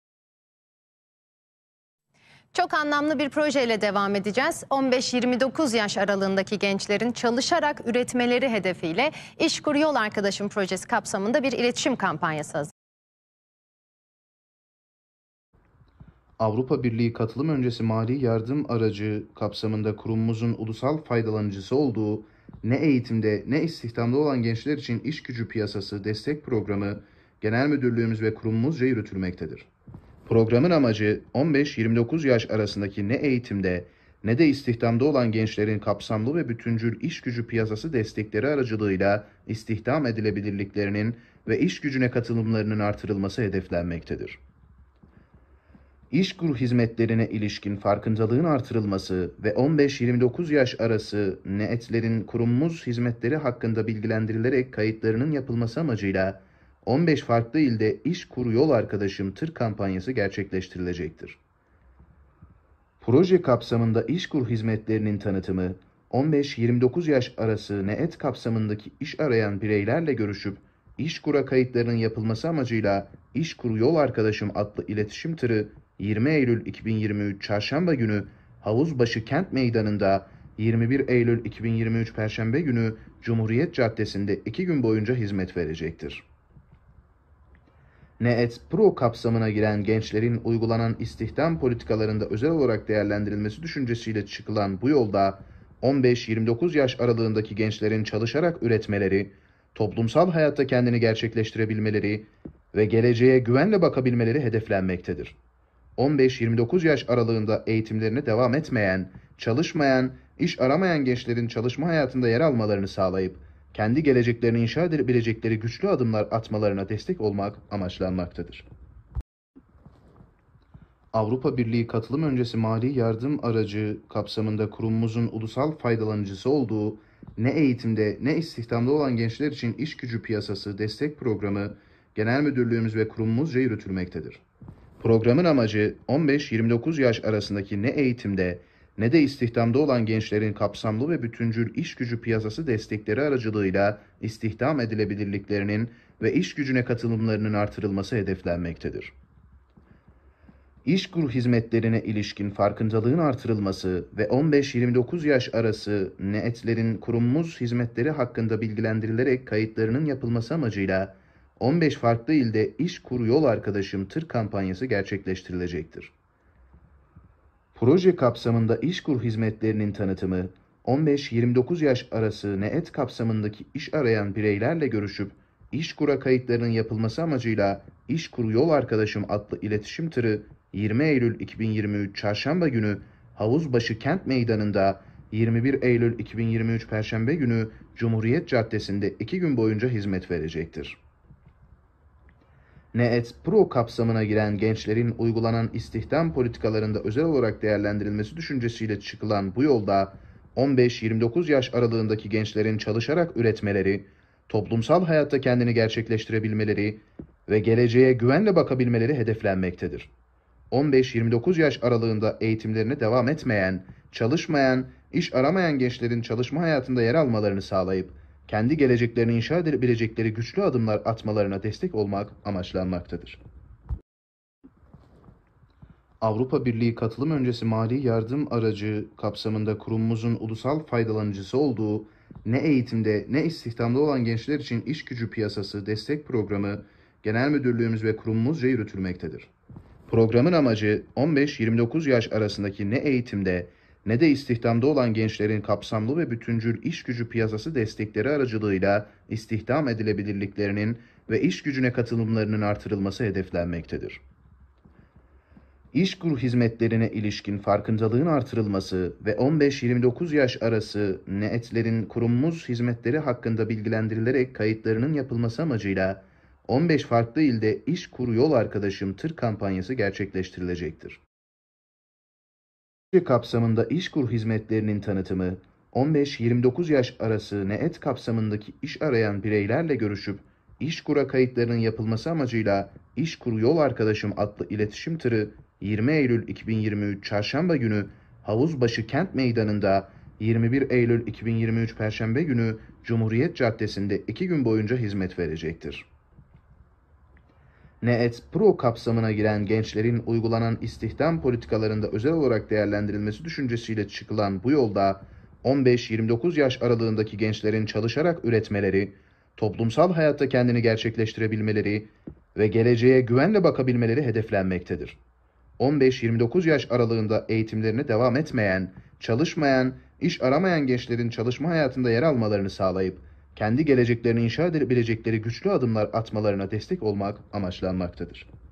“İŞKUR YOL ARKADAŞIM” TANITIM TIRI ERZURUM'DA başlıklı videonun kopyası
SARTV CEMİL SARITAŞ SHA Avrupa Birliği Katılım Öncesi Mali Yardım Aracı (IPA II) kapsamında Kurumumuzun ulusal faydalanıcısı olduğu “Ne Eğitimde Ne İstihdamda Olan Gençler İçin İşgücü Piyasası Destek Programı (NEET PRO)” Genel Müdürlüğümüz ve Kurumumuzca yürütülmektedir. Programın amacı, 15-29 yaş arasındaki ne eğitimde ne de istihdamda olan gençlerin kapsamlı ve bütüncül işgücü piyasası destekleri aracılığıyla istihdam edilebilirliklerinin ve işgücüne katılımlarının artırılması hedeflenmektedir.
İŞKUR hizmetlerine ilişkin farkındalığın artırılması ve 15-29 yaş arası NEET’lerin Kurumumuz hizmetleri hakkında bilgilendirilerek kayıtlarının yapılması amacıyla 15 farklı ilde “İŞKUR Yol Arkadaşım” tır kampanyası gerçekleştirilecektir.
 Proje kapsamında İŞKUR hizmetlerinin tanıtımı, 15-29 yaş arası NEET kapsamındaki iş arayan bireylerle görüşüp İŞKUR’a kayıtlarının yapılması amacıyla  “İŞKUR Yol Arkadaşım” adlı iletişim tırı 20 Eylül 2023 Çarşamba günü Havuz Başı Kent Meydanında 21 Eylül 2023 Perşembe günü Cumhuriyet Caddesinde 2 gün boyunca hizmet verecektir.
            NEET PRO kapsamına giren gençlerin uygulanan istihdam politikalarında özel olarak değerlendirilmesi düşüncesiyle çıkılan bu yolda 15 – 29 yaş aralığındaki gençlerin çalışarak üretmeleri, toplumsal hayatta kendini gerçekleştirebilmeleri ve geleceğe güvenle bakabilmeleri hedeflenmektedir.
          15-29 yaş aralığında eğitimlerine devam etmeyen, çalışmayan, iş aramayan gençlerin çalışma hayatında yer almalarını sağlayıp, kendi geleceklerini inşa edebilecekleri güçlü adımlar atmalarına destek olmak amaçlanmaktadır.